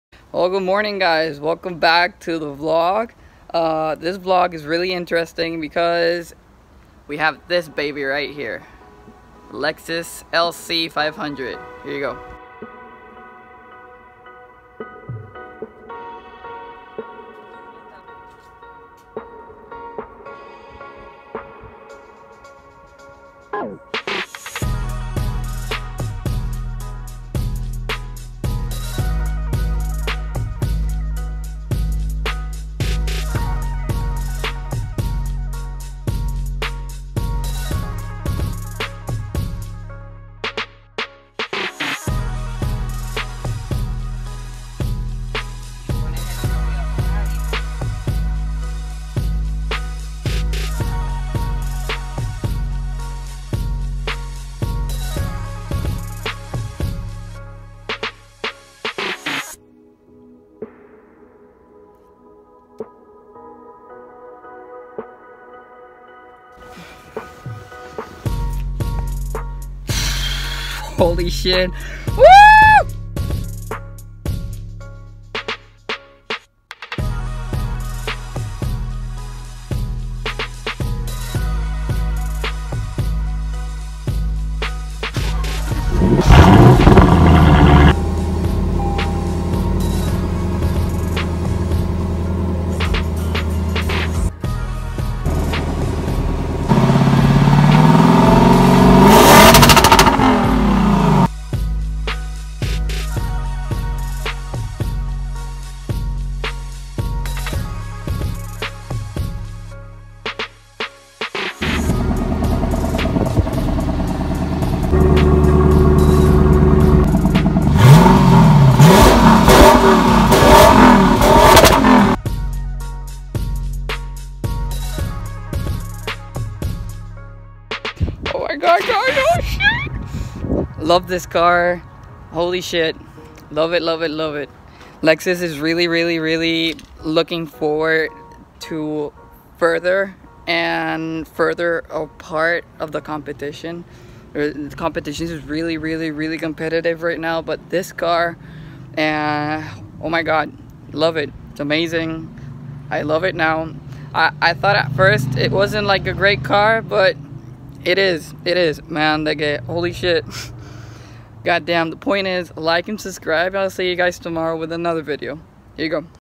Lexus LC 500 (Was gonna be Vlog #27)
This video was gonna be Vlog #27 but i wanted to wait a little bit more because wanted to be a surprise car for my viewers and my family. this Blog is about a lexus 2018 lc 500 overview of this amazing car.